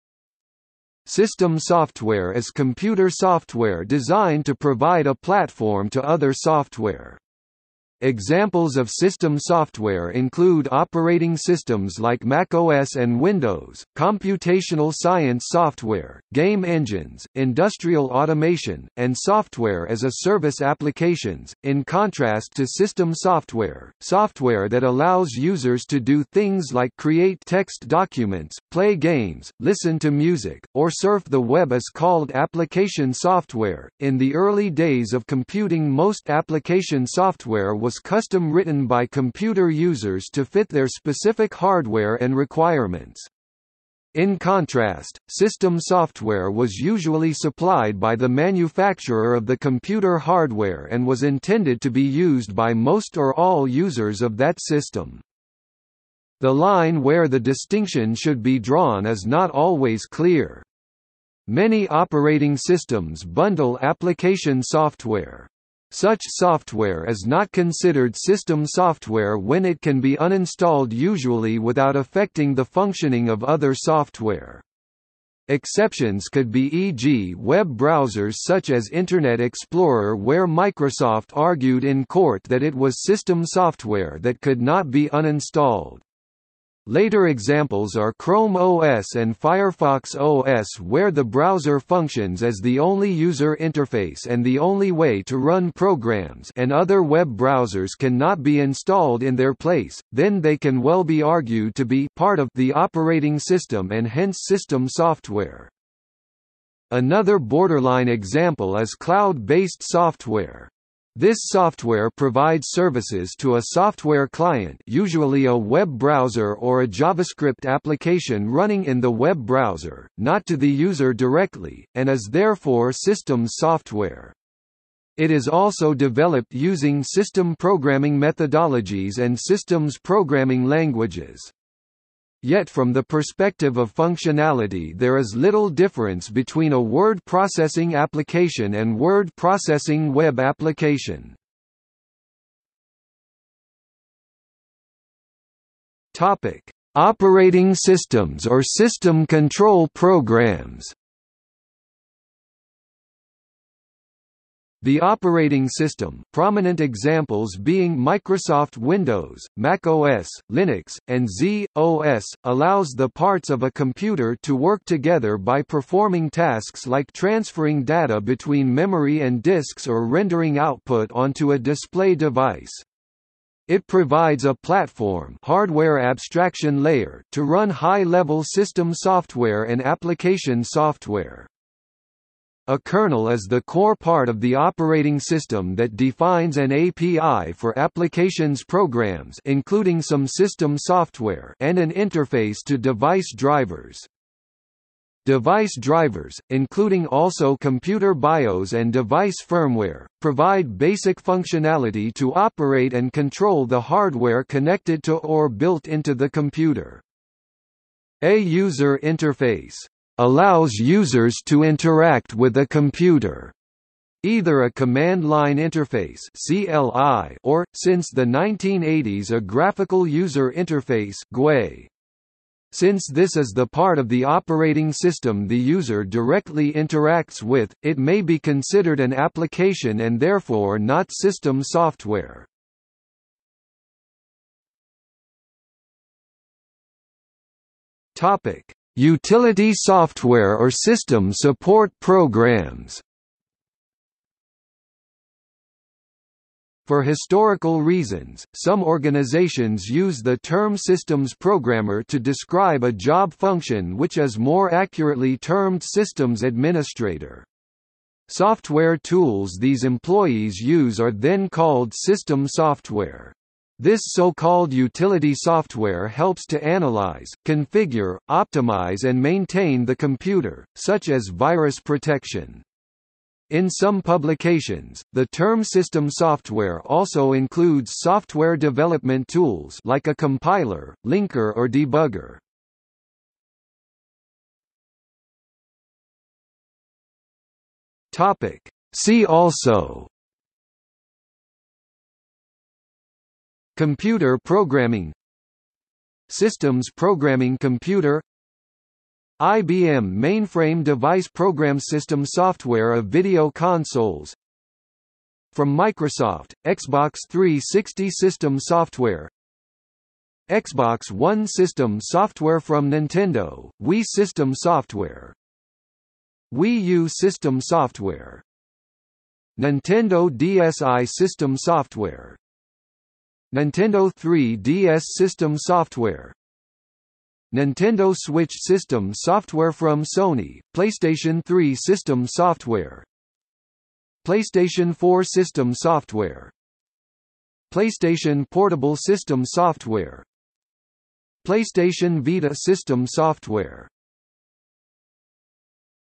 Systems software | Wikipedia audio article
00:02:22 1 Operating systems or system control  programs
00:03:54 2 Utility software or system support programs
00:04:44 3 See also

- Socrates

SUMMARY
System software is computer software designed to provide a platform to other software. Examples of system software include operating systems like macOS and Windows, computational science software, game engines, industrial automation, and software as a service applications.In contrast to system software, software that allows users to do things like create text documents, play games, listen to music, or surf the web is called application software.In the early days of computing most application software was custom-written by computer users to fit their specific hardware and requirements. In contrast, system software was usually supplied by the manufacturer of the computer hardware and was intended to be used by most or all users of that system.
The line where the distinction should be drawn is not always clear. Many operating systems bundle application software. Such software is not considered system software when it can be uninstalled usually without affecting the functioning of other software. Exceptions could be e.g. web browsers such as Internet Explorer where Microsoft argued in court that it was system software that could not be uninstalled. Later examples are Chrome OS and Firefox OS where the browser functions as the only user interface and the only way to run programs (and other web browsers can not be installed in their place), then they can well be argued to be (part of) the operating system and hence system software.
Another borderline example is cloud-based software. This software provides services to a software client (usually a web browser or a JavaScript application running in the web browser), not to the user directly, and is therefore systems software. It is also developed using system programming methodologies and systems programming languages. Yet from the perspective of functionality there is little difference between a word processing application and word processing web application